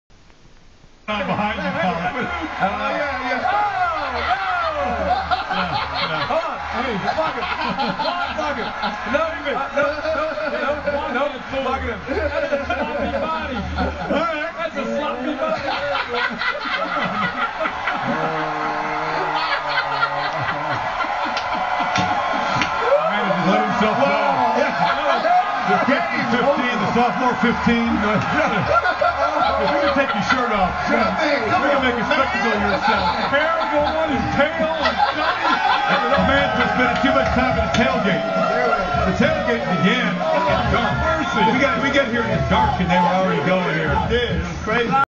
ESPN Funniest Commentary Ever
Sorry about the bad quality but check the guy with the shirt off in the background that's what they were laughing at